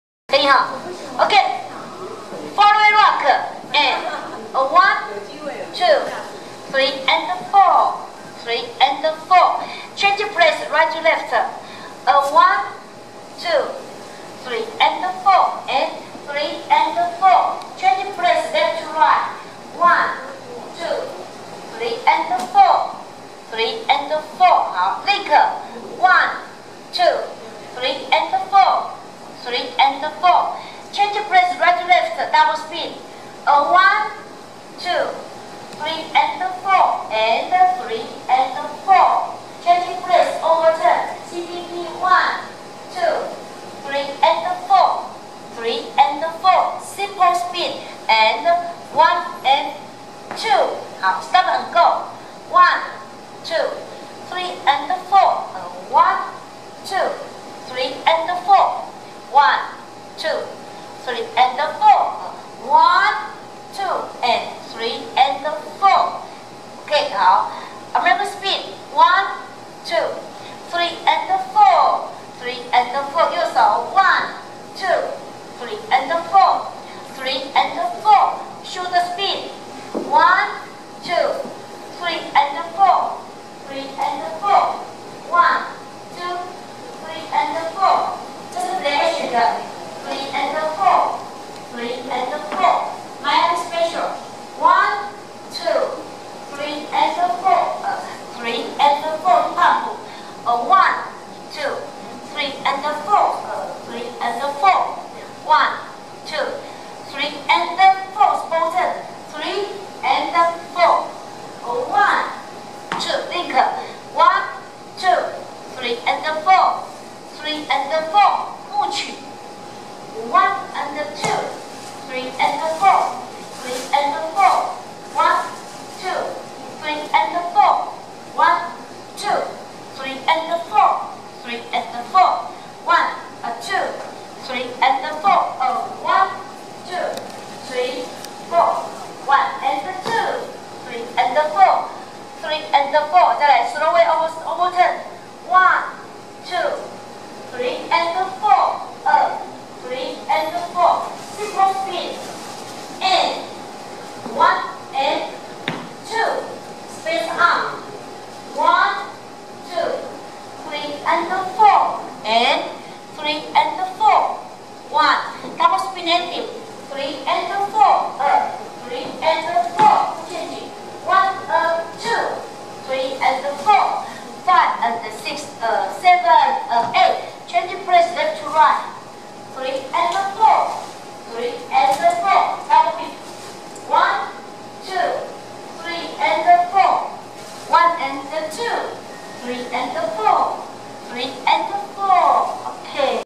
2016 Jive 捷舞中級舞步（快八步）蔡秀慧老師 雙人示範 couple demonstration ISTD
Jive Couple dance demonstration ISTD. 蔡秀慧老師 週六捷舞 2016 雙人示範 ，上課地點：台北市永康街60號4樓，逢週六下午1:30~3:00拉丁班，又要開新課了，蔡老師 Tel： 。. Fallaway Rock, Change of Places, Link, Change of Places Double Spin, COP overturned, Stop and Go, American Spins, Shoulder Spin, Miami Special, Hip Bumps, Mooch, Fallaway Throwaway Overturned, Simple Spin, Spanish Arms, Double spin, Flick- ball-changes, Double Whip.